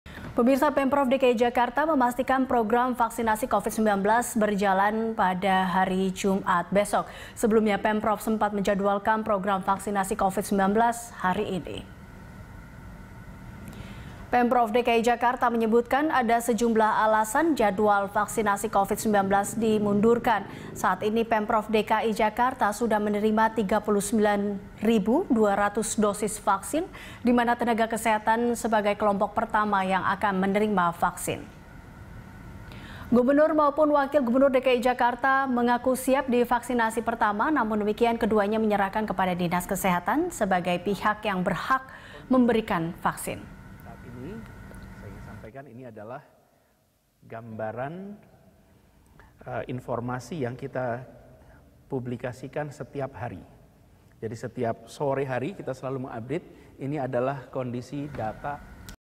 Pemprov DKI Jakarta Undur Jadwal Vaksinasi Corona Besok
Pemprov DKI Jakarta memastikan program vaksinasi Covid-19 berjalan pada Jumat (15/1) besok. Sebelumnya, Pemprov sempat menjadwalkan program vaksinasi Covid-19 hari ini.